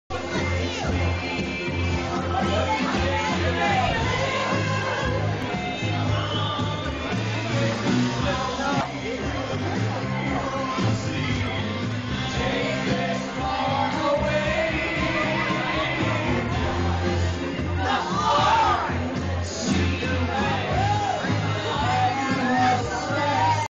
Superhero Night Floorshow - Completely Crazy VID 20120729 00218
Wally performs Brad as The Greatest American Hero for Completely Crazy's Superhero Night at the Hollywood Blvd.